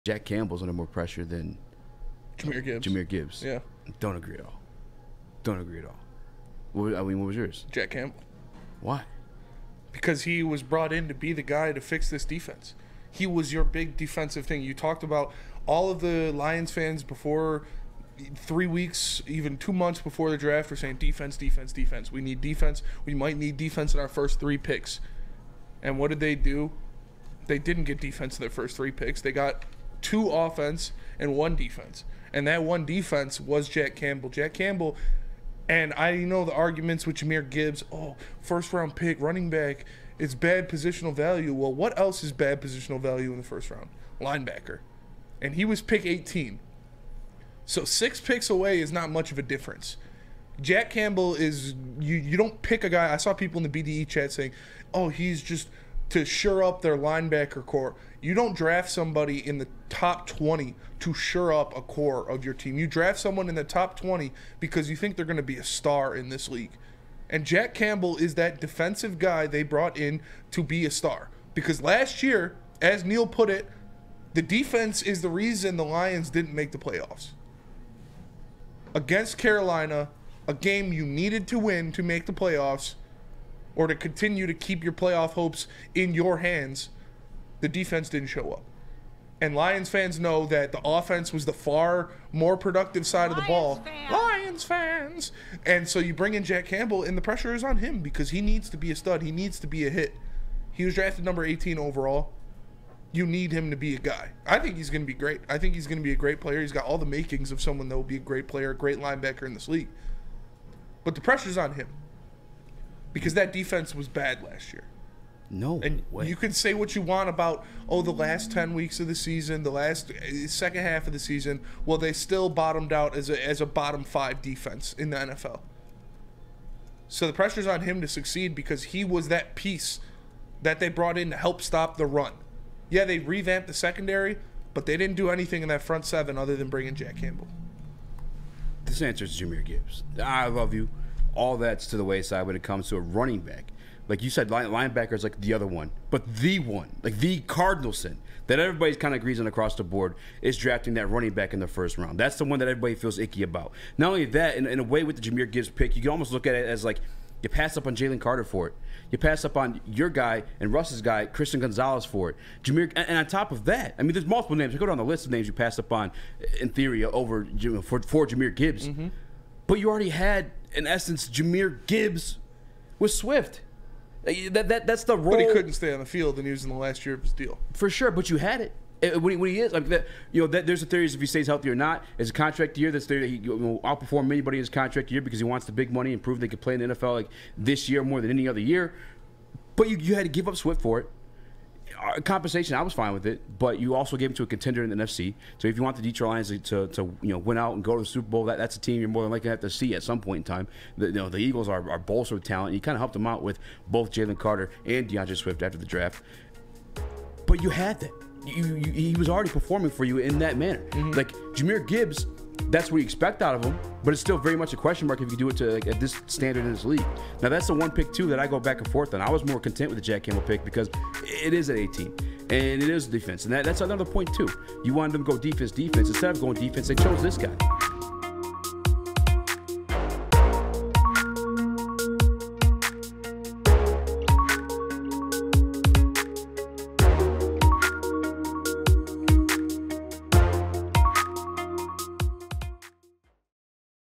Detroit Lions Rookies Jack Campbell Vs. Jahmyr Gibbs!
Spencer Raxter and Eazy debate on which Detroit Lions rookie is under the most pressure, Jahmyr Gibbs or Jack Campbell.